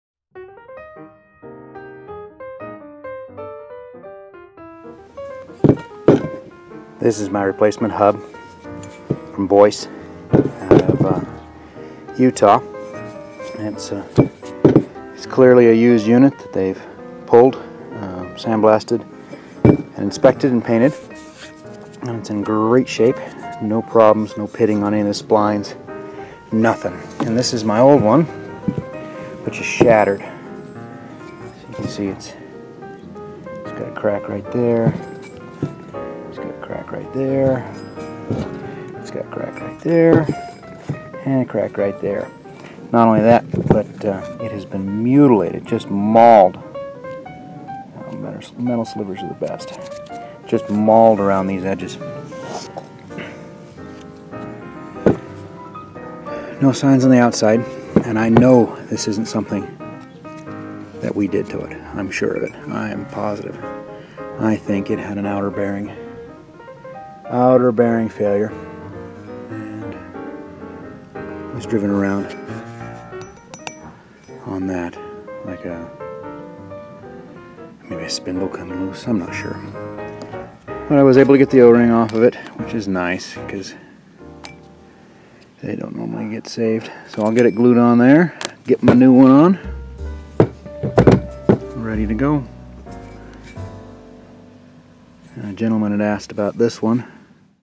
Inspecting the New Hubs
Ordered a replacement 5 ton Rockwell hub from Boyce Equipment in Ogden Utah.